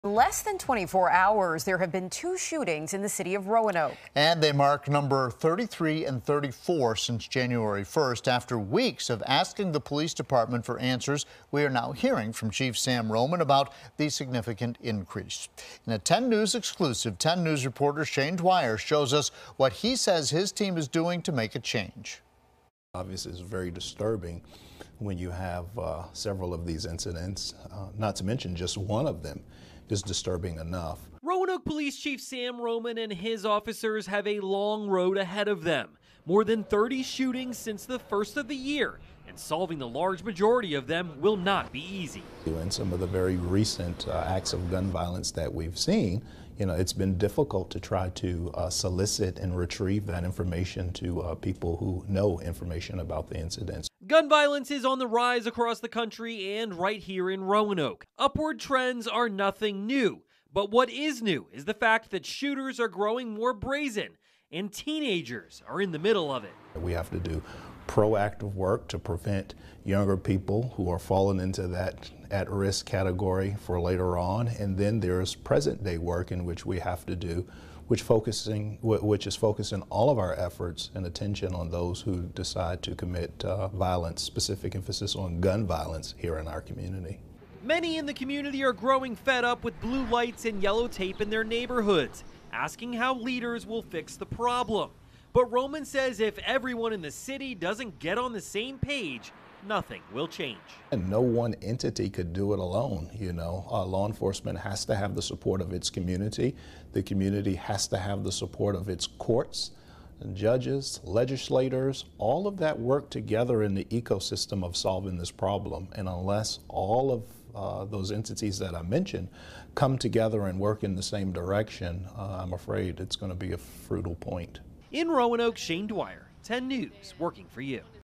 2 Roanoke shootings in less than 24 hours
In less than 24 hours, there have been two shootings in the City of Roanoke.